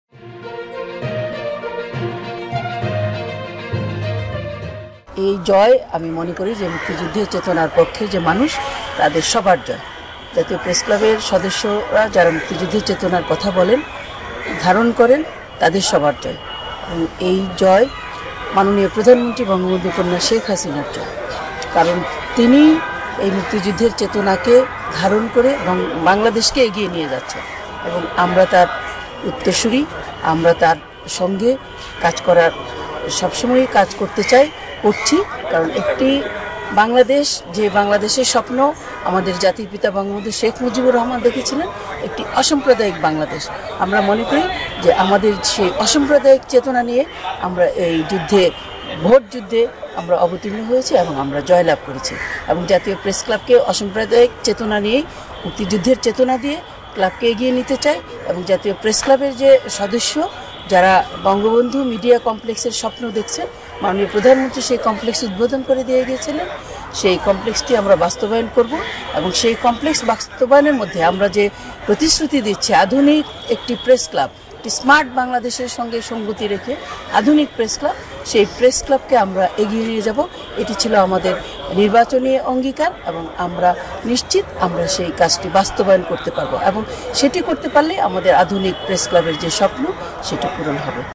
অসাম্প্রদায়িক মুক্তিযুদ্ধের চেতনায় আধুনিক প্রেসক্লাব তৈরি করা হবে - নবনির্বাচিত প্রেসক্লাব সভাপতি
ব্রেকিং নিউজ :- সমগ্র বাংলাদেশে দৈনিক ঐশী বাংলায় ভিডিও সংবাদদাতা নিয়োগ চলছে, যোগাযোগ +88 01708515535